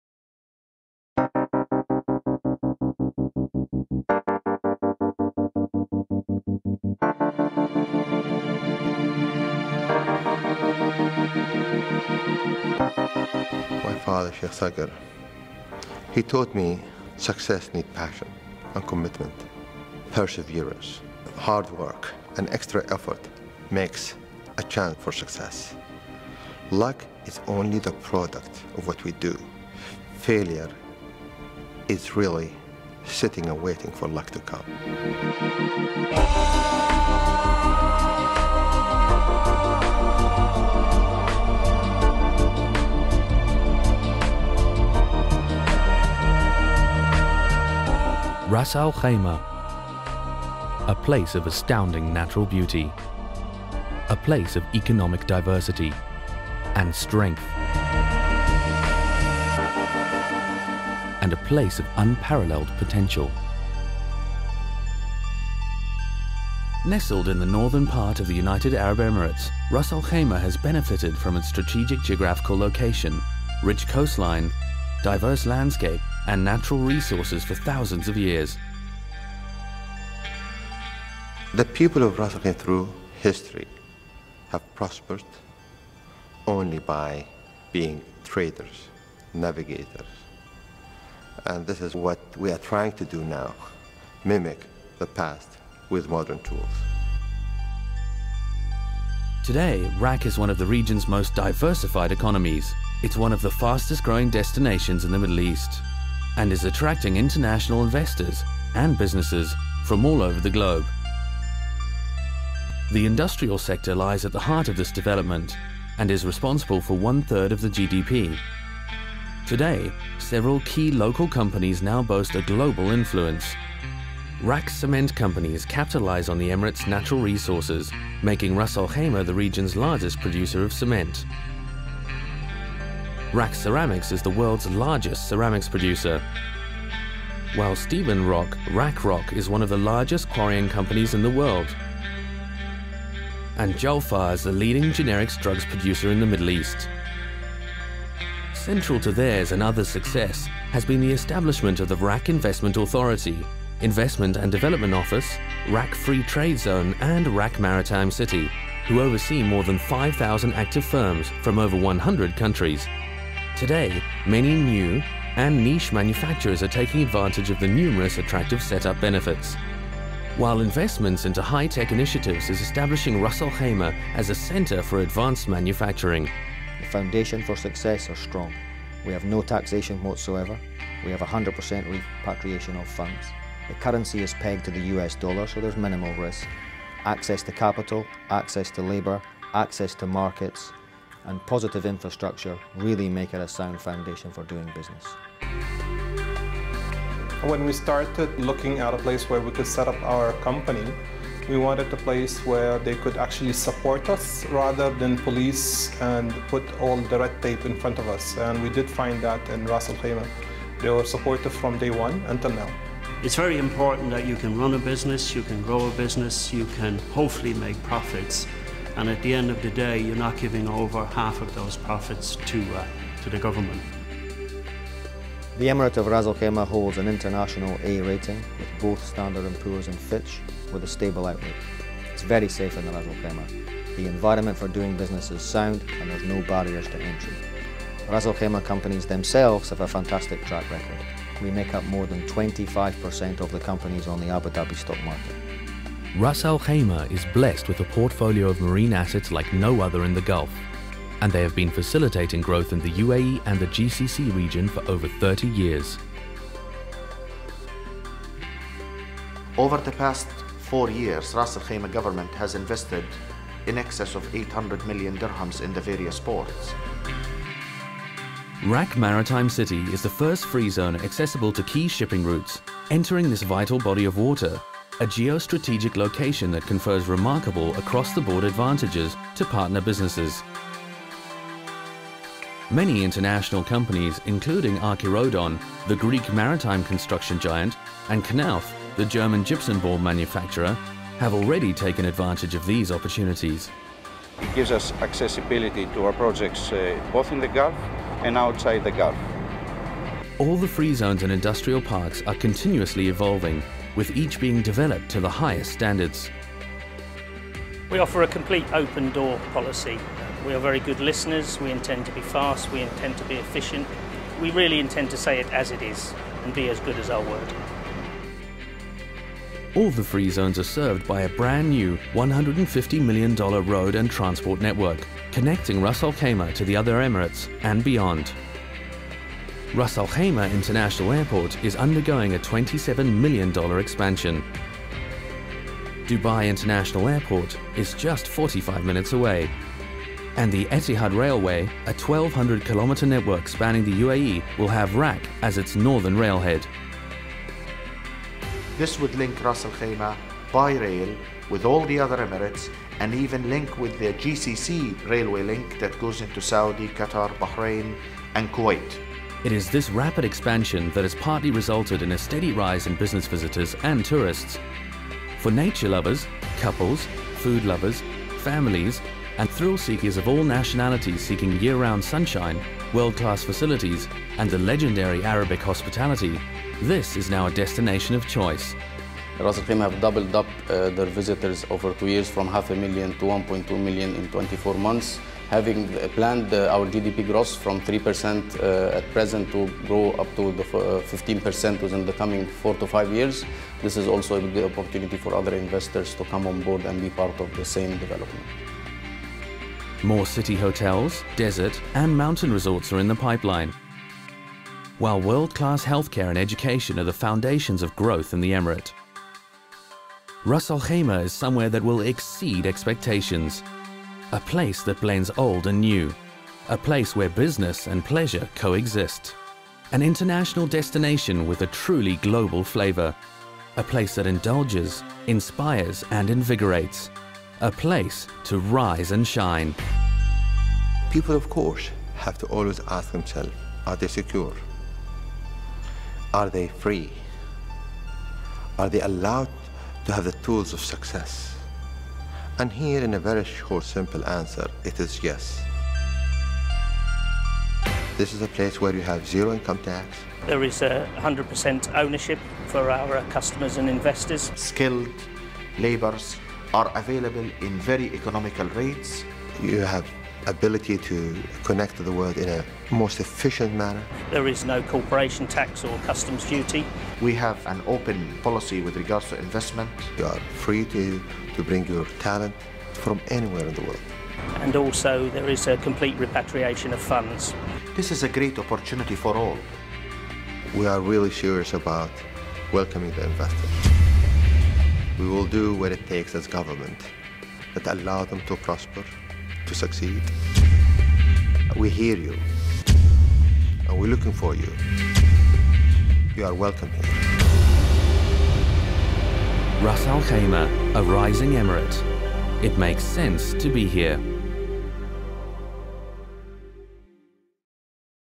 Ras Al Khaimah Overview with Sheikh Saud bin Saqr Al Qasimi
Sheikh Saud bin Saqr Al Qasimi, Supreme Counsel Member introduces Ras Al Khaimah. Provides an overview of Ras Al Khaimah companies including RAK Cement Company, RAK Ceramics, and Julphar generic drugs. Introduces the RAK Investment and Development Office, RAK Free Trade Zone, Maritime City, Saqr Port, Al Jazeera Port and RAK Khor Port. Includes information on RAK tourism, education, and health. Sheikh Saud bin Saqr Al Qasimi discusses the benefits of RAKs tax-free environment and infrastructure.